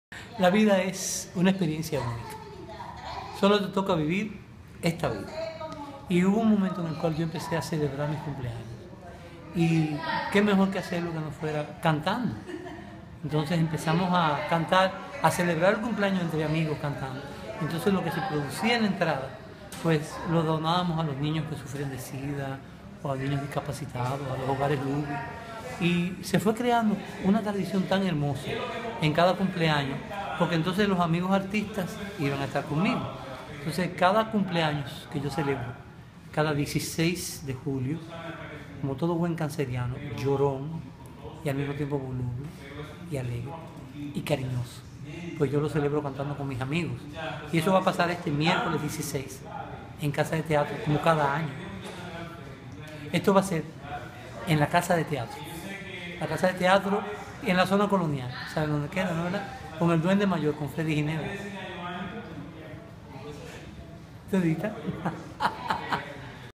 Claudio Cohen
Explica por qué celebra su cumpleaños cantando... 17 julio 2014.